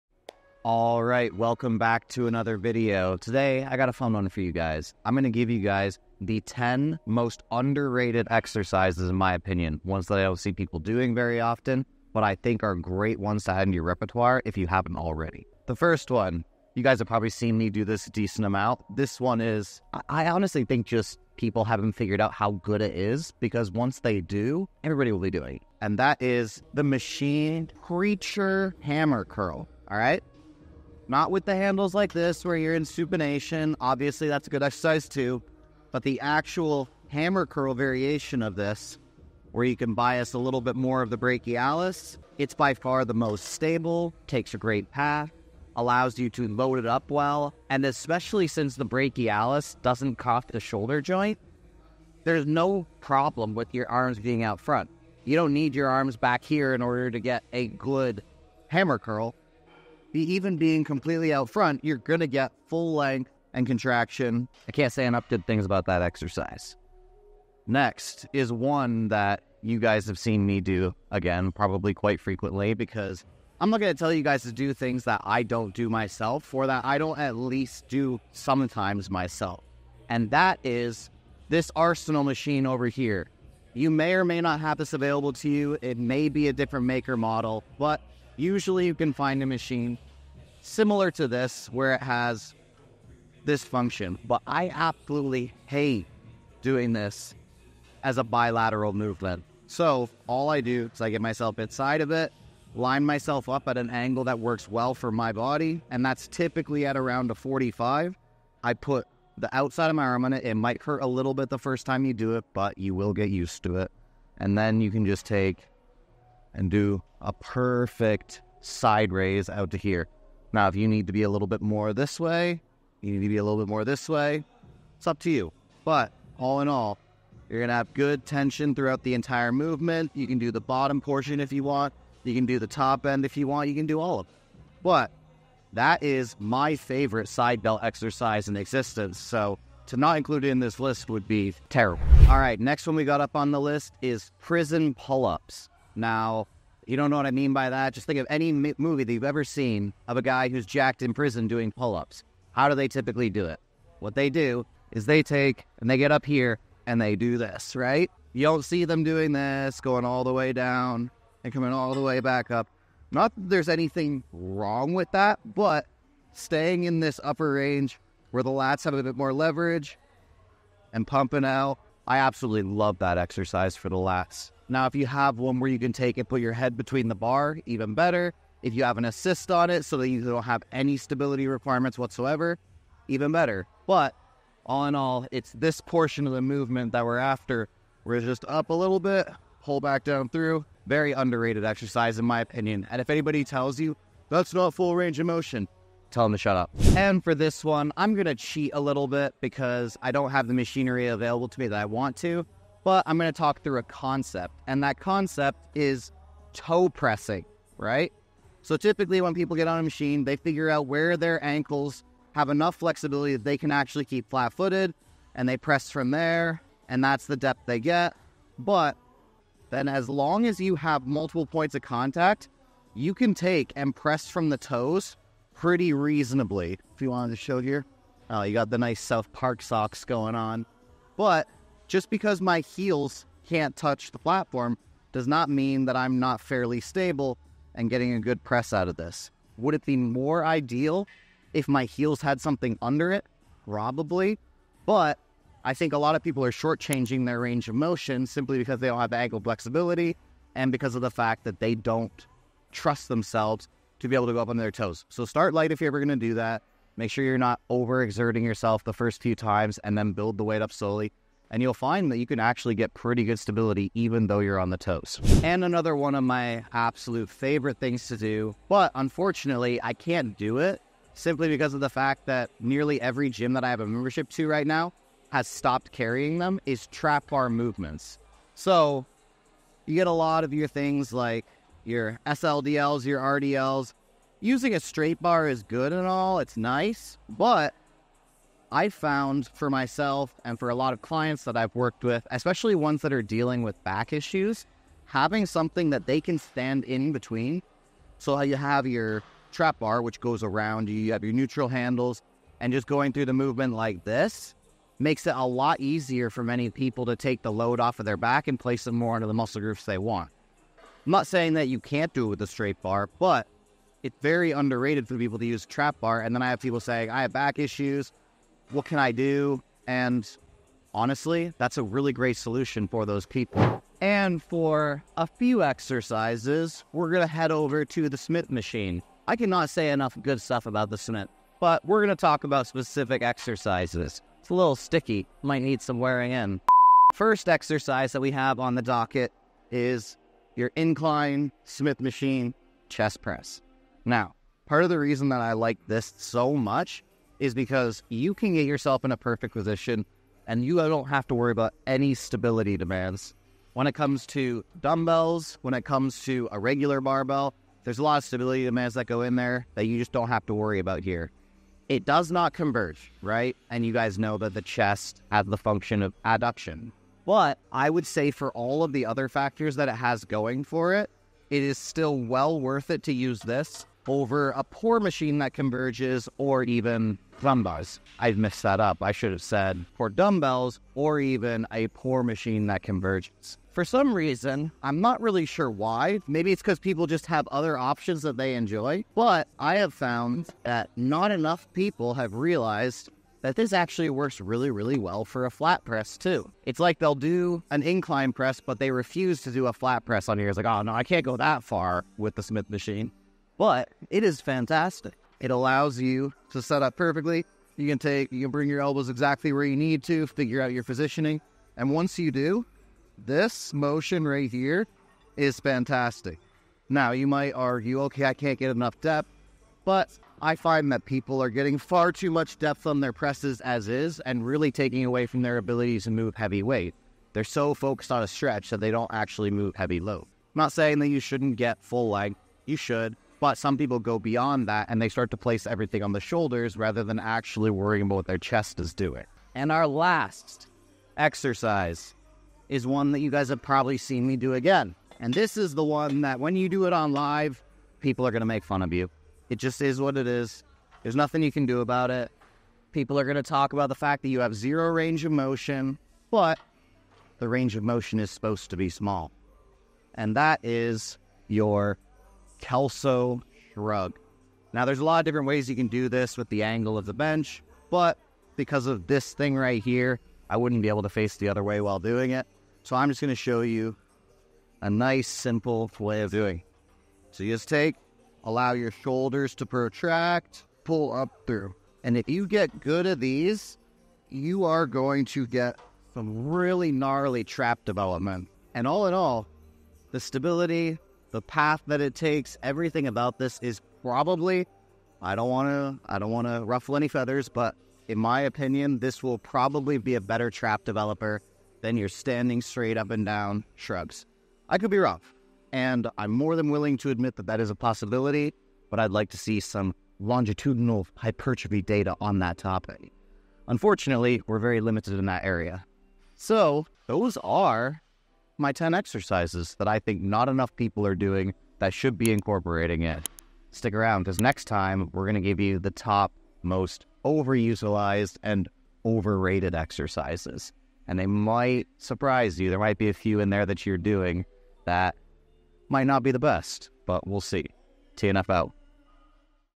10 Exercises You AREN'T Doing But Should Be
Obviously no exercise is necessary as there are substitutions for nearly every movement/muscle group. But these are exercises that I think are highly undervalued for what they provide. If you're looking for a change of pace, or even a confirmation on some of the stuff you're currently doing, give a few of these (or all of them) a go.
TNF out.